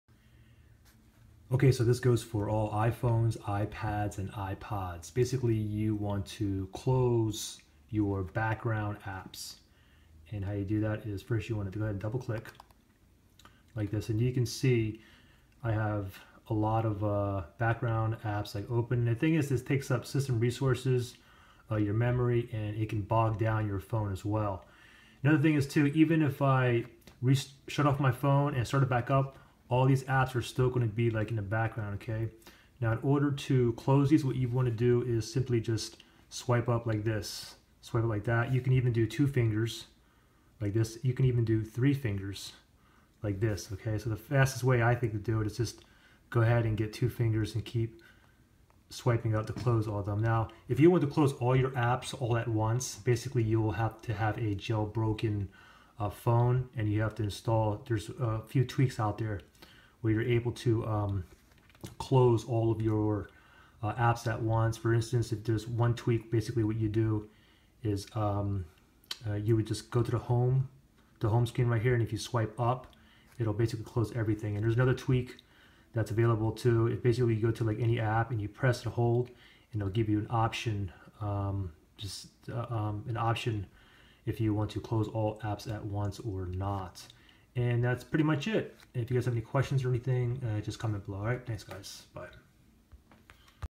2016: Close Background Running Apps for iPhone/iPads/ iPods
This goes for all iphones, ipads, and ipods running iOS 7 and above. If you open an app then close it is still running in the background. Even if you restart your iphone it will still be running in the background. Too many apps running in the background can cause your iOS device to become sluggish and laggy not to mention take up mobile data and drain your battery.